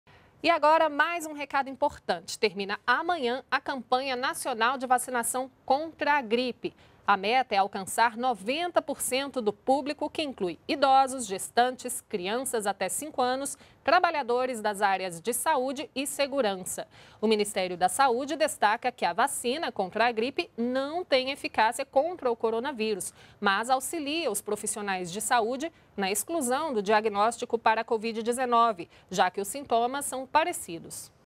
Vacinação contra a gripe termina amanhã (30)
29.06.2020 - A meta é alcançar 90% do público que inclui idosos, gestantes, crianças até cinco anos, trabalhadores das áreas de saúde e segurança.